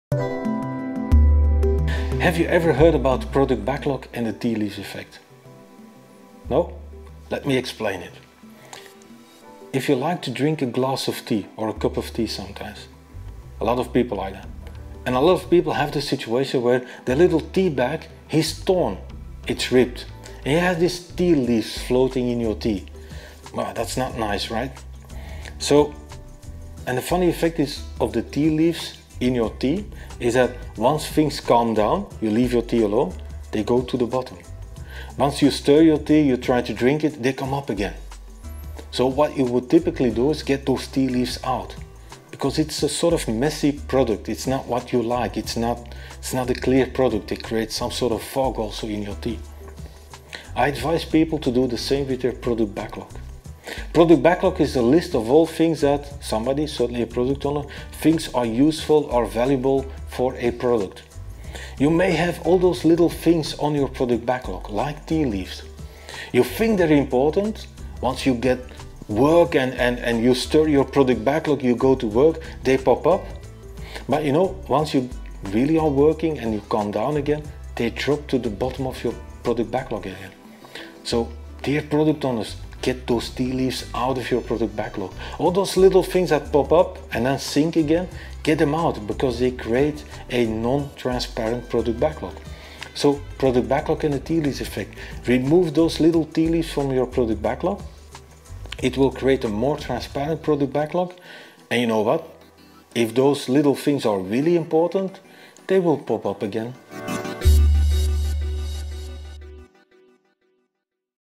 Product Backlog and the Tea Leaves Effect
Gunther Verheyen advises Product Owners in Scrum to be aware of the tea leaves effect on Product Backlog, because the effect reduces the transparency of this artefact.

Gunther is an independent Scrum Caretaker; a connector, writer, speaker, humanizer.